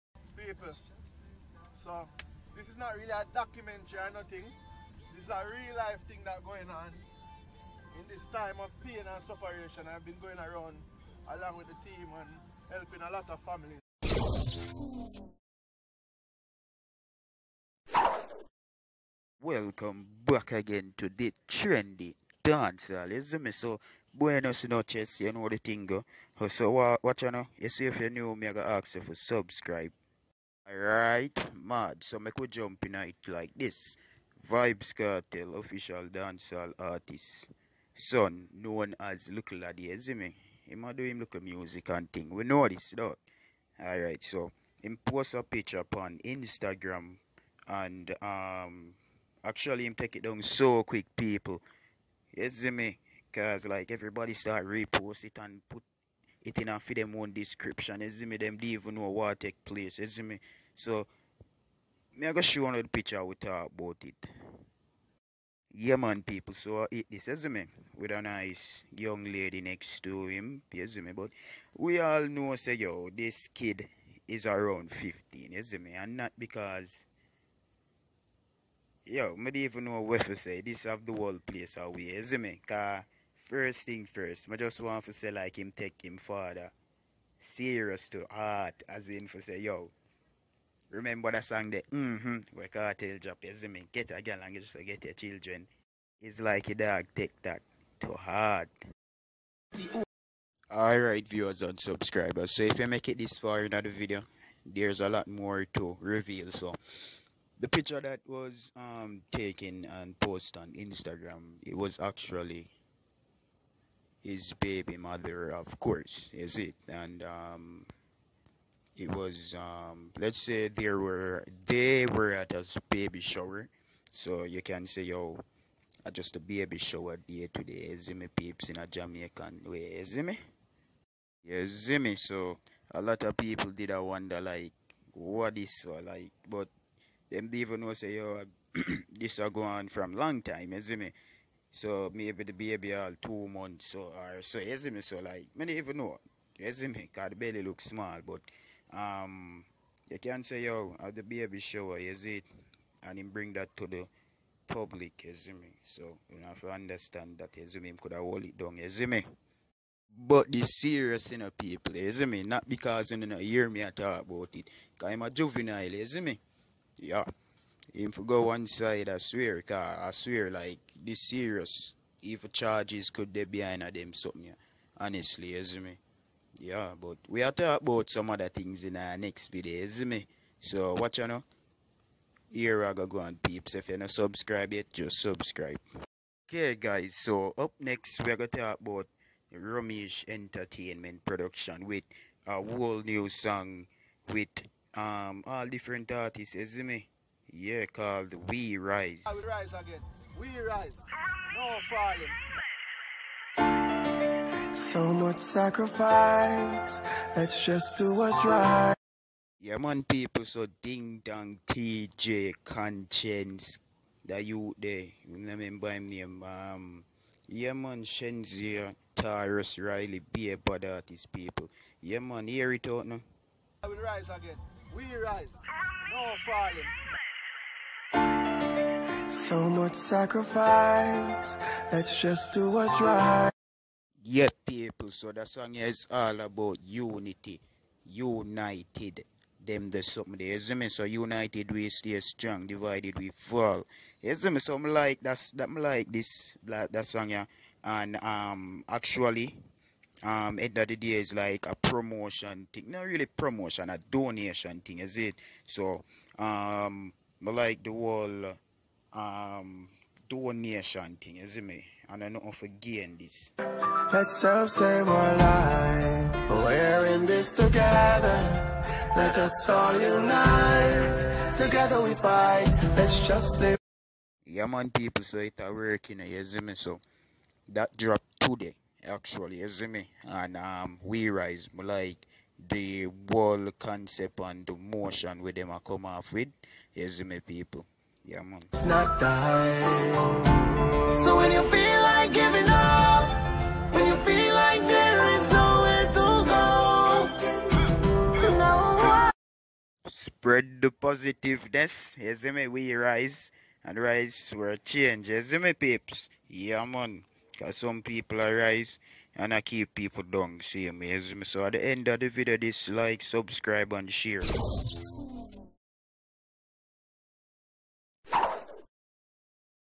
BREAKING NEWS | VYBZ KARTEL Son Likkle Addi Baby Mother | We Rise Teejay & Romeich Ent
Likkle Addi Turn Vybz Kartel In A Grandfather